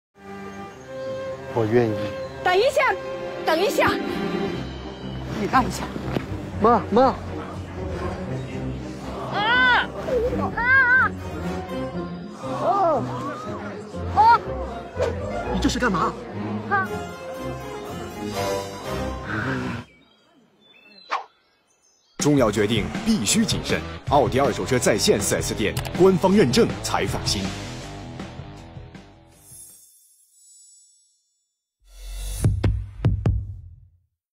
Audi's controversial TV ad in mainland China
The ad comparing a soon-to-be wed bride with shopping for a new car has drawn a heated response from social media users in mainland China, with people across the country outraged by the imagery in the ad, calling it an insult to women and a huge step backward for women’s rights. Full story